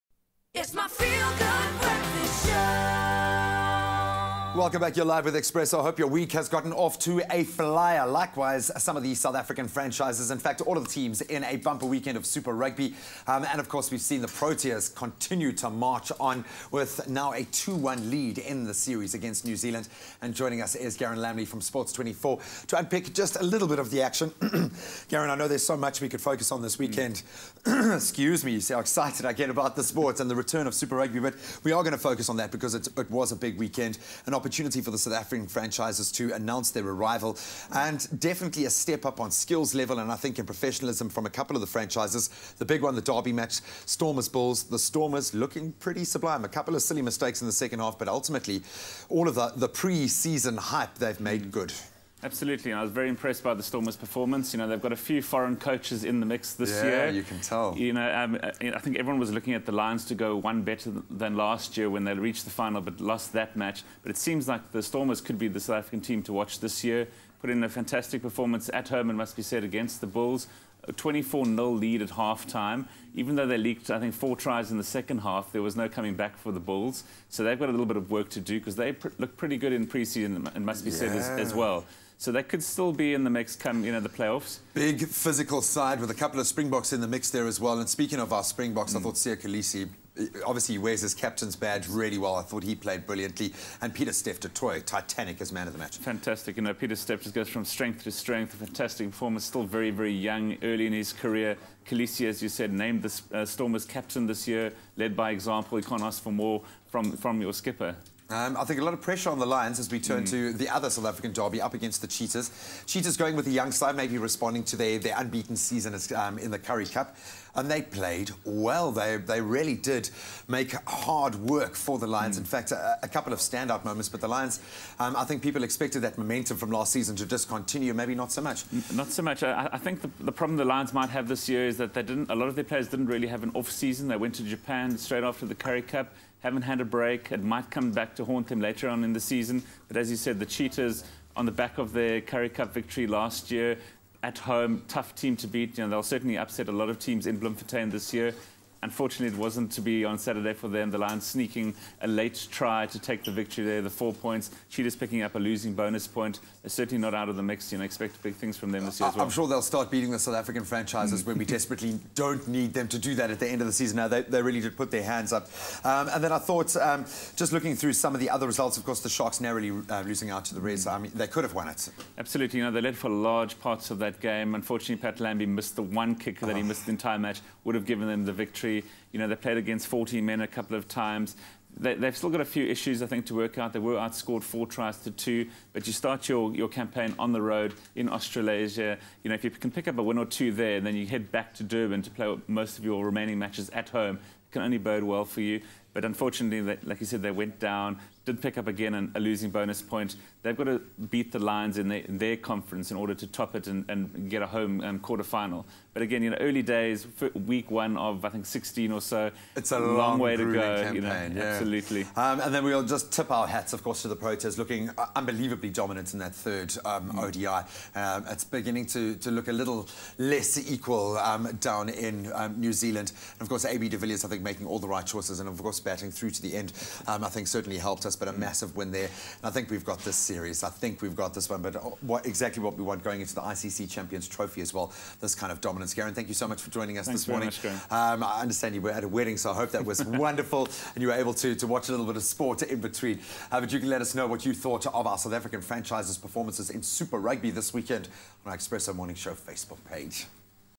Sports Weekend Review with Garrin Lambley
A review of selected sports results from the weekend with Sports24 editor Garrin Lambley